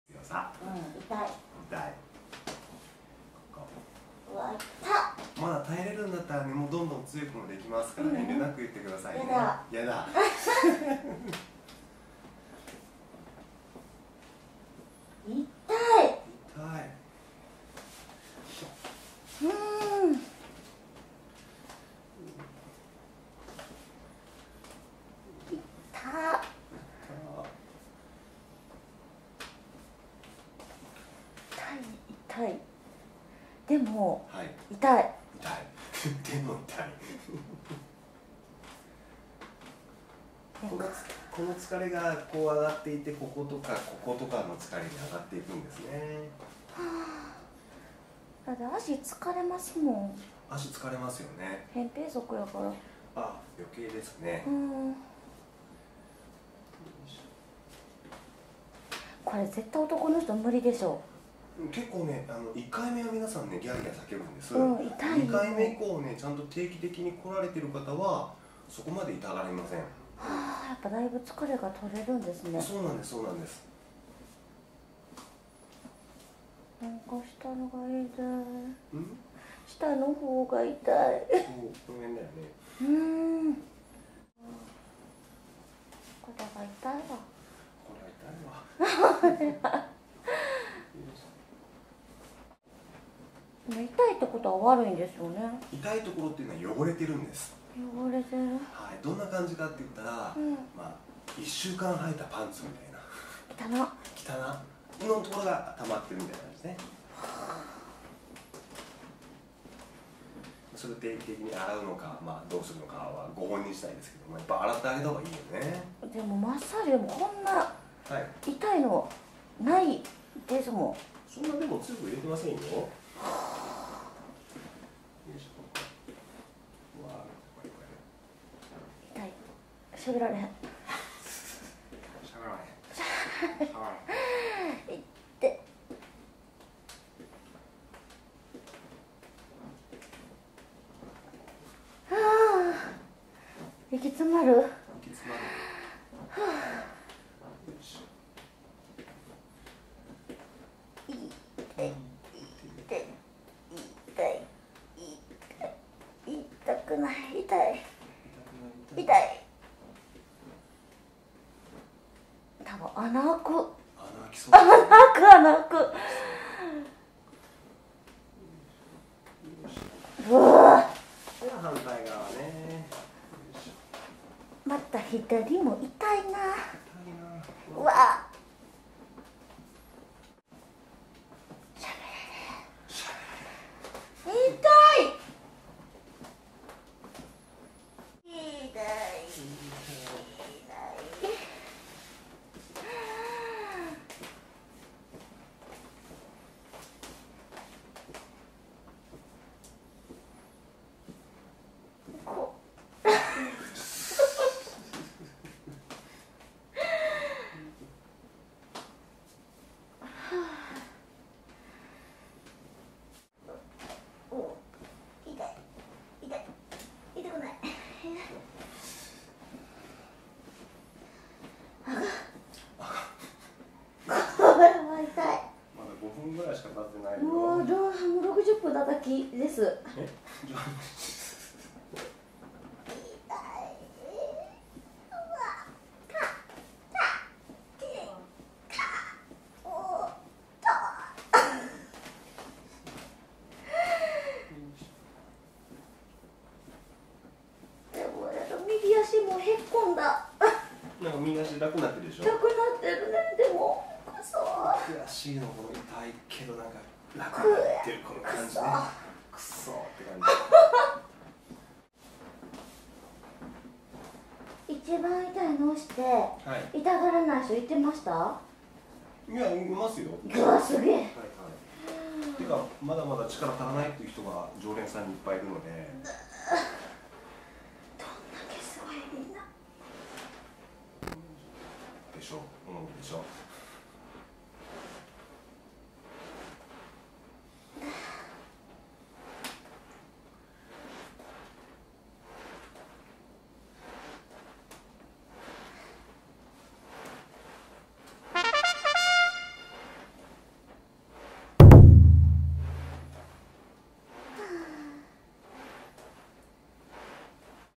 【痛い整体】痛い？痛くない！？悶絶整体を受けて叫んでいる動画。
ふくらはぎの施術で痛がっている悶絶整体の動画になります。
今回の方は、反応が関西のノリなので楽しんでもらえるのではないでしょうか？
足の疲れや、足のむくみ、膝痛み、などが気になる方は参考になると思います

epark経由での割引予約はできない様になりました。ご理解ください。

新着順